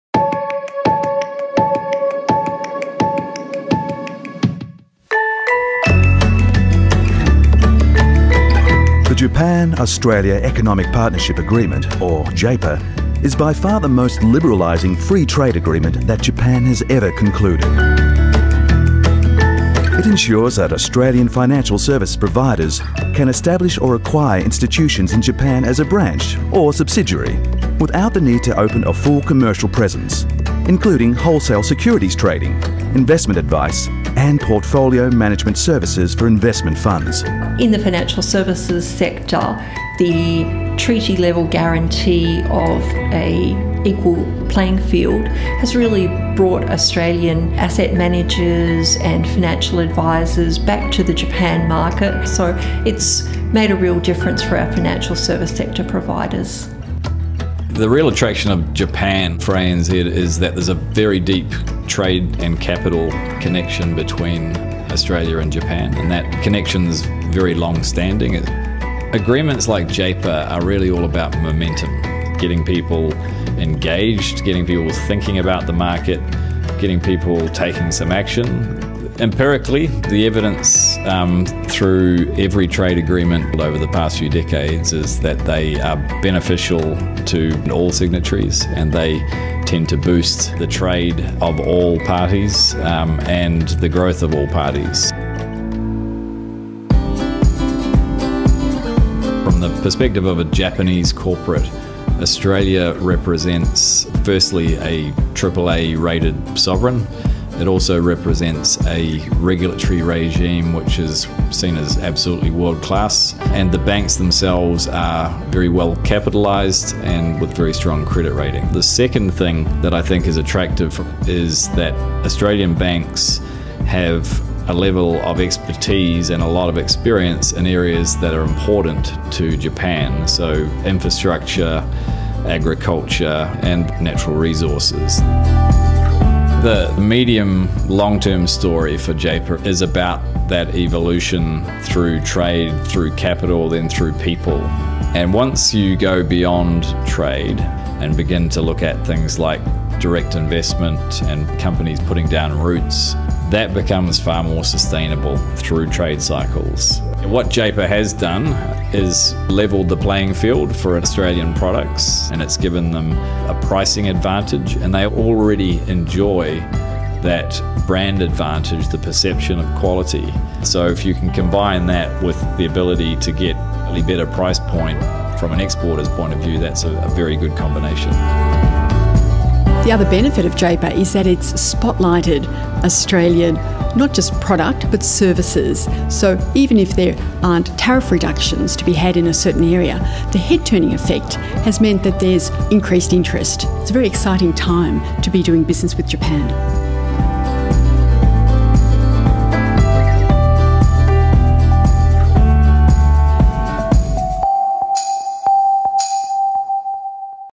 A video case study on Australian financial services in Japan
See how the Japan-Australia Economic Partnership Agreement is helping Australian financial services firms.